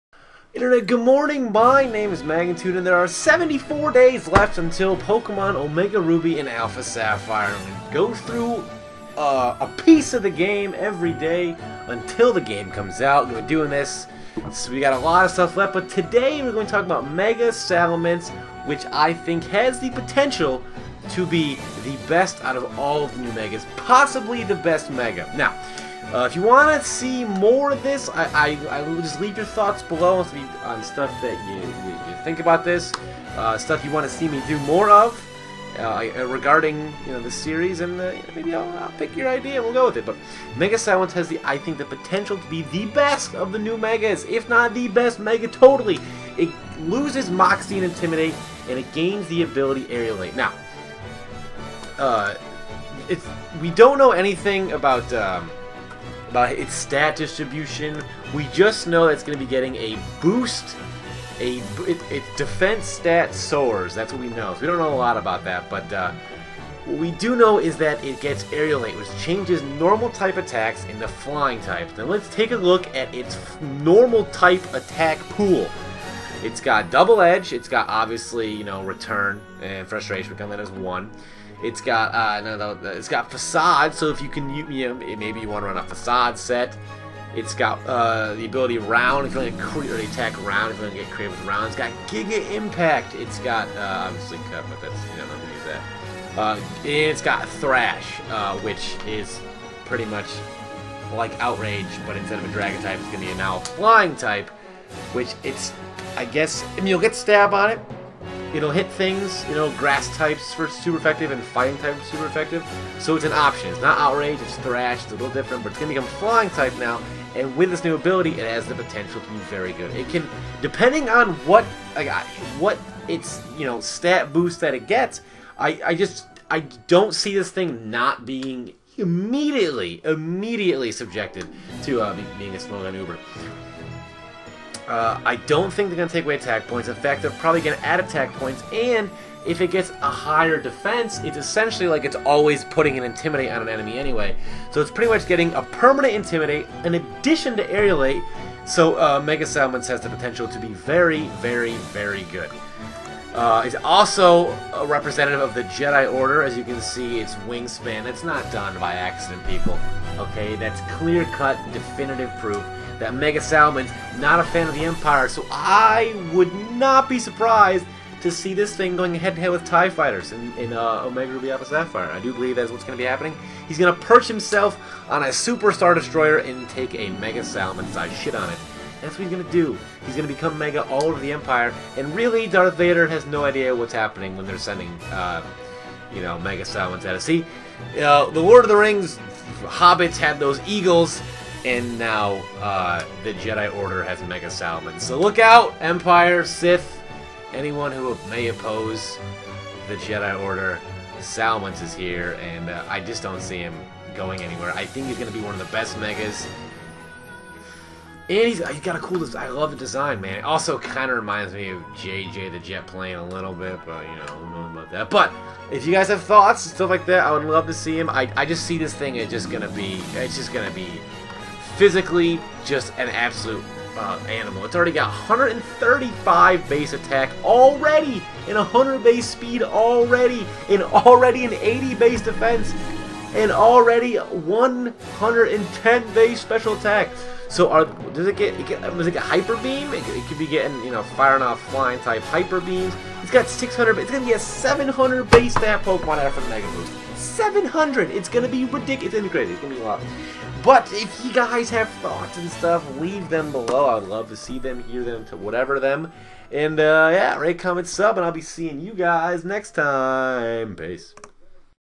Mega Salamence - Countdown to Pokemon [Omega Ruby/Alpha Sapphire] - 72 Days
72 Days remain until Pokemon Omega Ruby/Alpha Sapphire! Today, we are talking about the Jedi Order's newest weapon: Mega Salamence. This guy gets a Defense boost and honestly, I see him as one of the best megas to date. Enjoy!